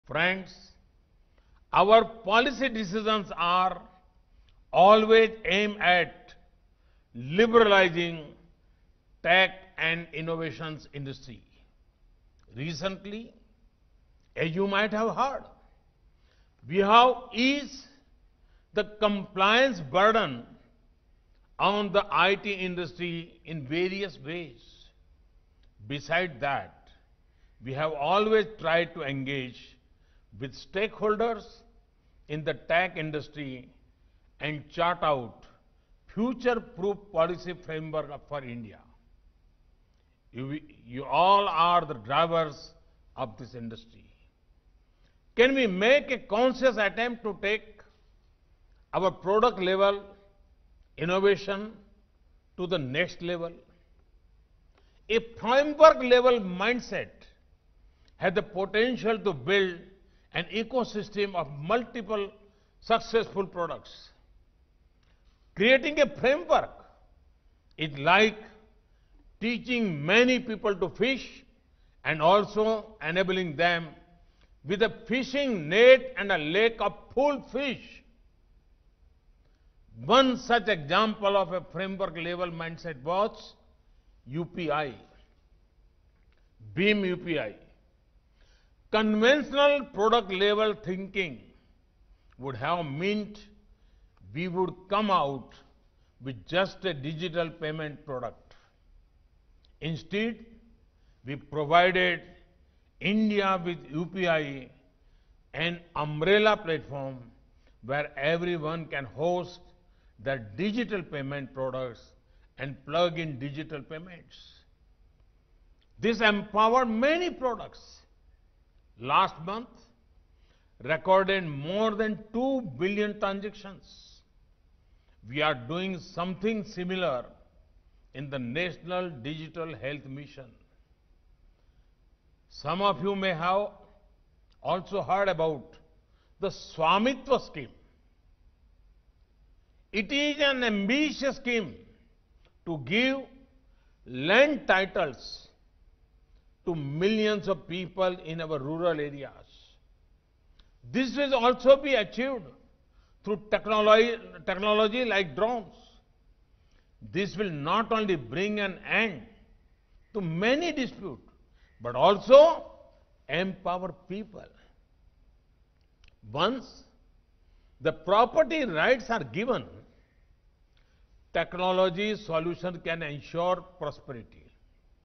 Our policy decisions are always aimed at liberalising tech and innovations industry: PM
Prime Minister Narendra Modi virtually inaugurates Bengaluru Technological Summit 2020